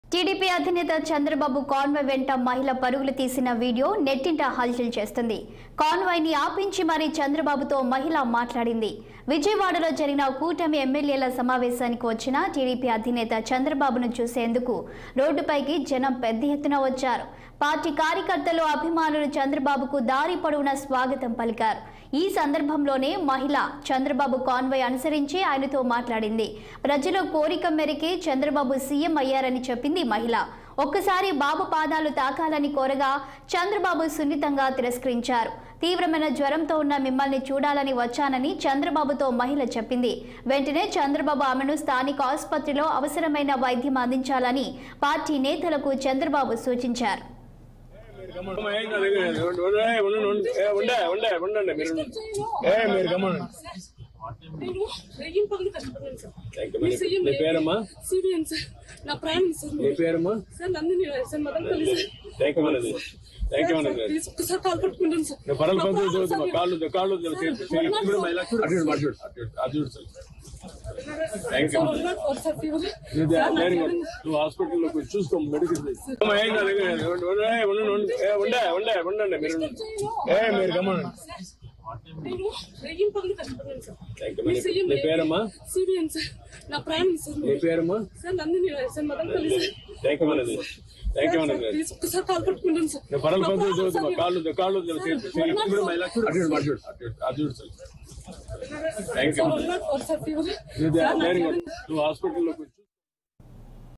Lady Fan Ran Along with Convoy To Meet Chandrababu
చంద్రబాబు కాన్వాయ్ వెంట మహిళ పరుగులు
 CBN పేరుతో పచ్చబొట్టు..షాక్ లో బాబు